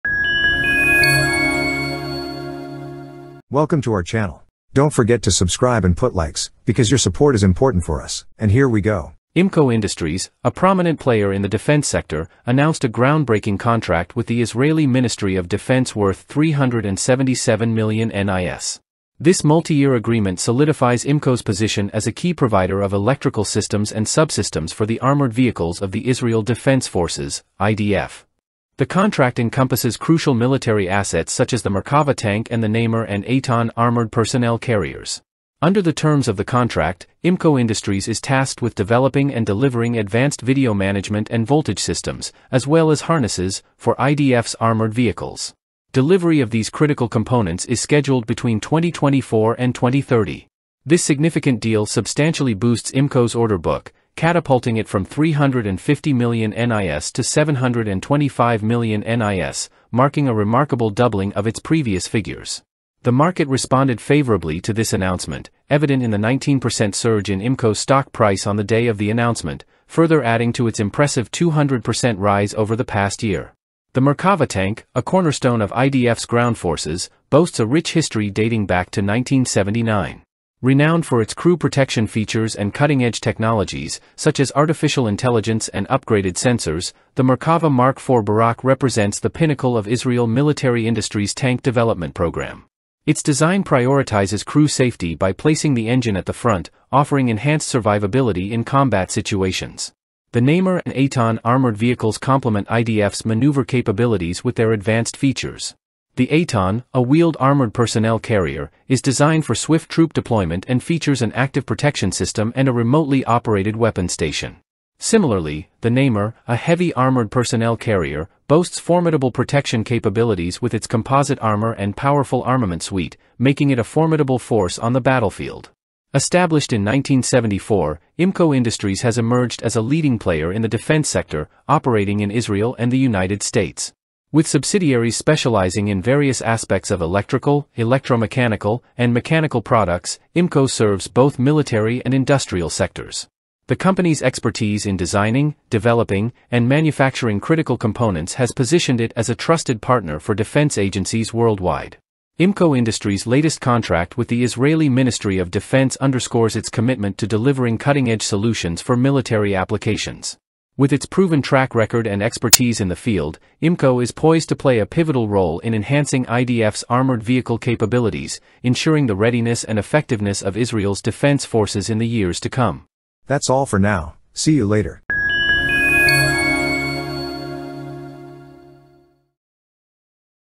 Israel Contracts Imco Industries to Modernise Armored Vehicles Including Merkava Tank
Imco Industries, a prominent player in the defense sector, announced a groundbreaking contract with the Israeli Ministry of Defense worth 377 million NIS. This multi-year agreement solidifies Imco's position as a key provider of electrical systems and subsystems for the armored vehicles of the Israel Defense Forces (IDF). The contract encompasses crucial military assets such as the Merkava tank and the Namer and Eitan armored personnel carriers.